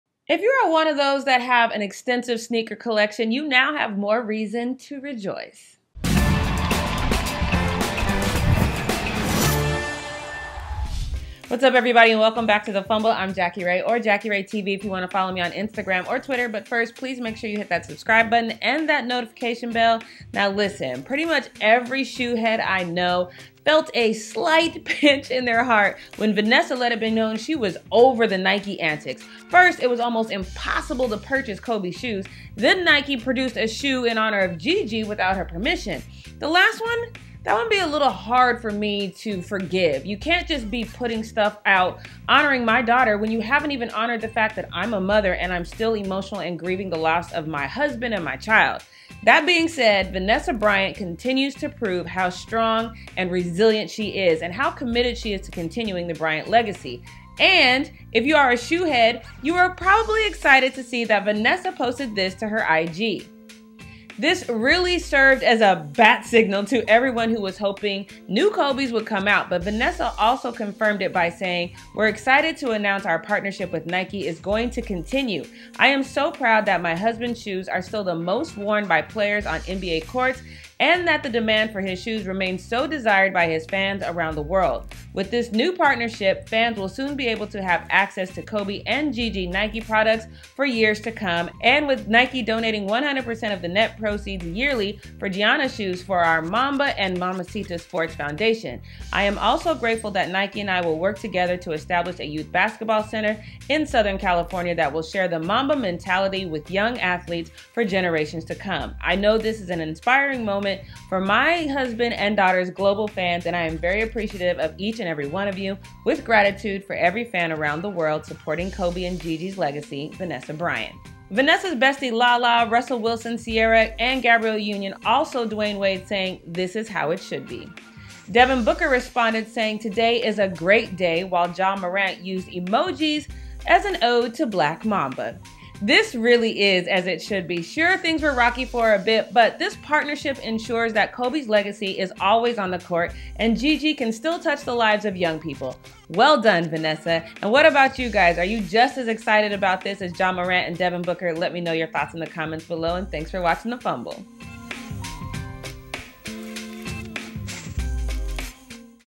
Vanessa Bryant BREAKS INTERNET With Kobe x Nike IG Post
Vanessa Bryant continues to prove how strong and resilient she is, and how committed she is to continuing the Bryant legacy.  And, if you ARE a shoe head you were probably excited to see that Vanessa posted this to her IG.